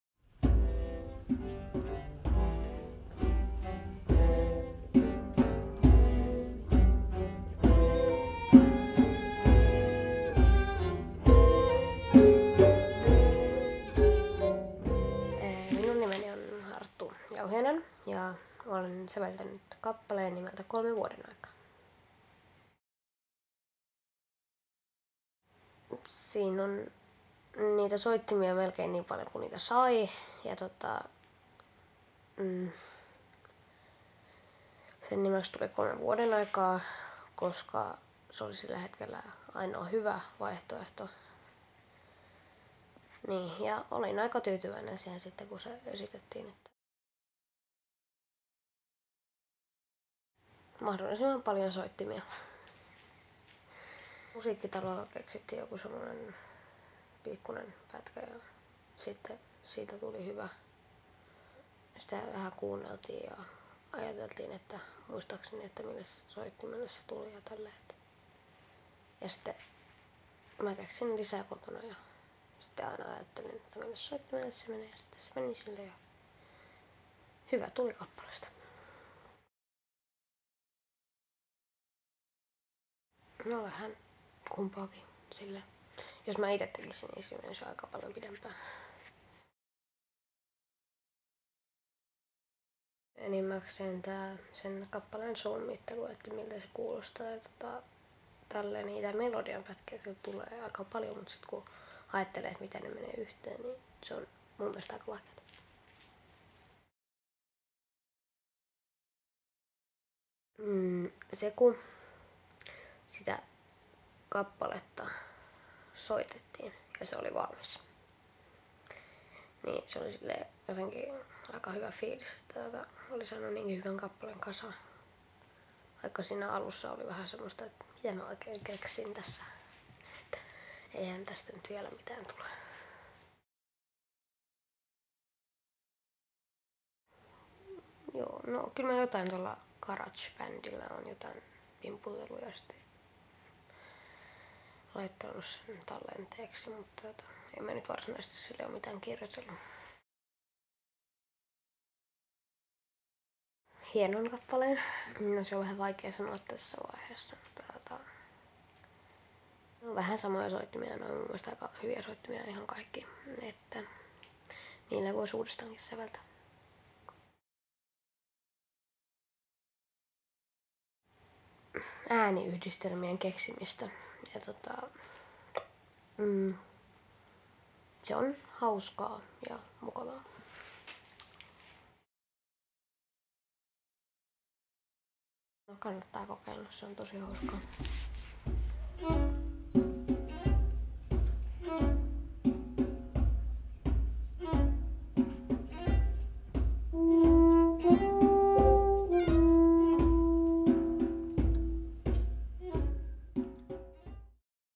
Arttu Jauhiaisen haastattelu
Arttu Jauhiainen kertoo haastattelussa säveltämisestä ja Kuule, minä sävellän! -projektista.

Minkälaisia terveisiä Arttu lähettää säveltämisestä kiinnostuneille lapsille: "Kannattaa kokeilla, se on tosi hauskaa."